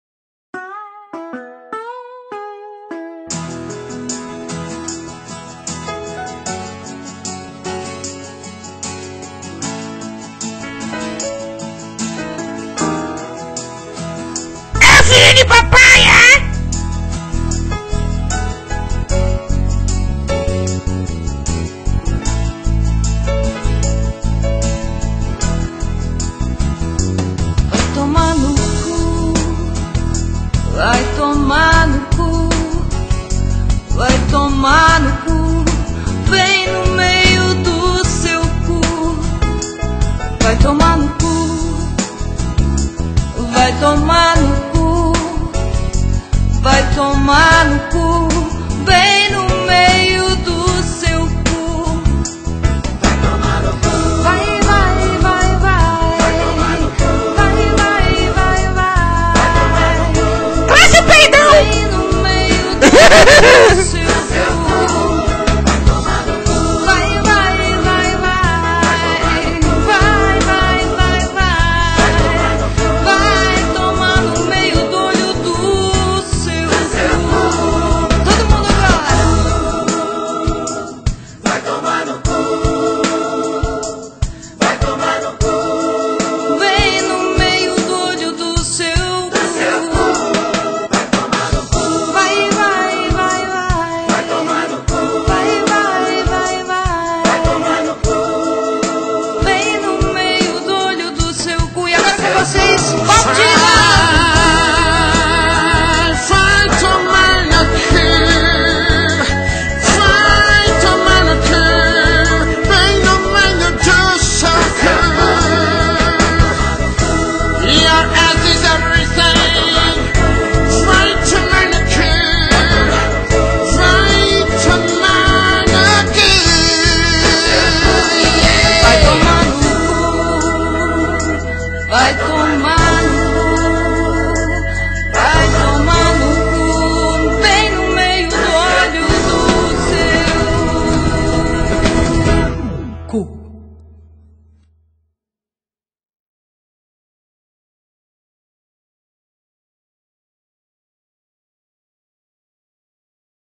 A Galera do Setor A
Video q eu fiz da galera do setor A zuando um pokin e com trilha sonora sugestiva.
(DE VOLTA !)